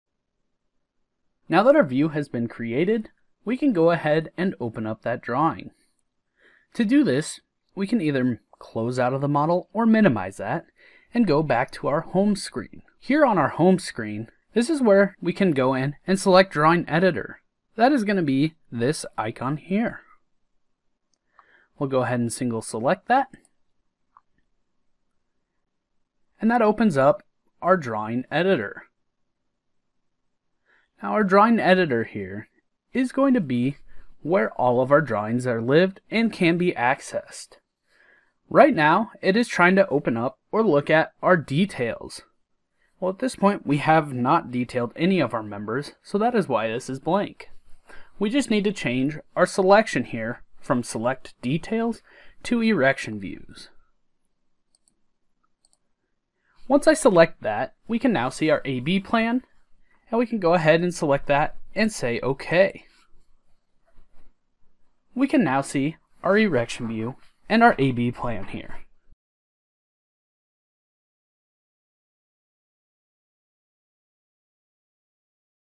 SDS 2022 Getting Started: Open Erection View Drawings
In this video, you will learn how to open erection views inside of Drawing Editor.

Upgrade today to experience our most intuitive and accessible release yet.